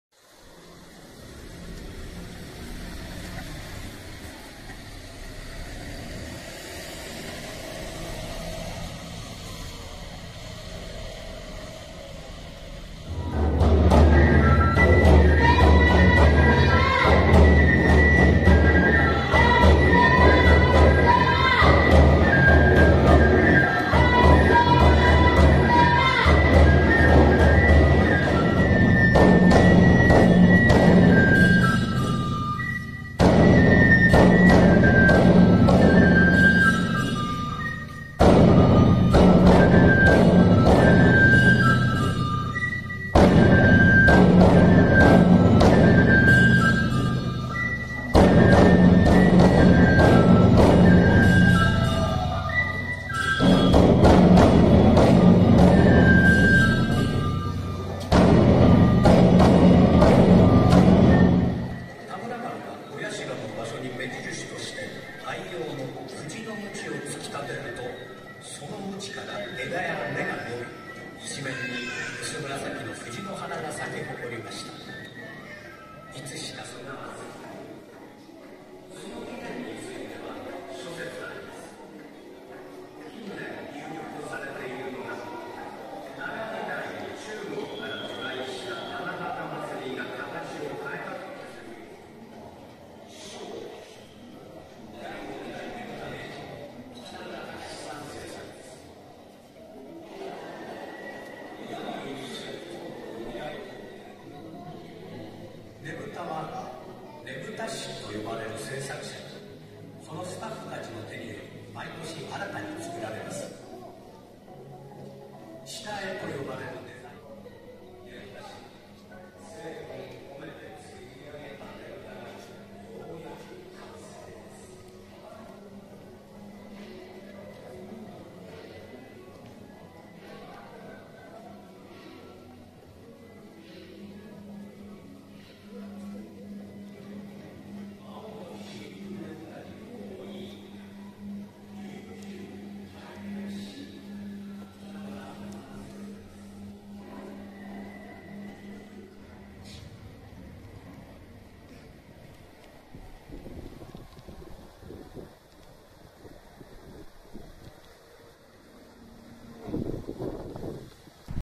新しく入れ替えたワラッセの大型ねぶた 2022.10.29
10月29日 ねぶたの家 ワ・ラッセにて 新しく入れ換えた展示大型ねぶたを撮影。

ワラッセの展示大型ねぶたは、毎年ねぶた祭が終わると受賞したねぶたの何台かを展示しています。 そのため、その年の前のねぶたを入れ替えるため何度来てもそのねぶたを近くで見れます。また、時間を調べると、お囃子の実演も見ることもできます。

展示しているねぶたの詳細はこちら↓
・龍王①（今年のねぶた大賞 ）
④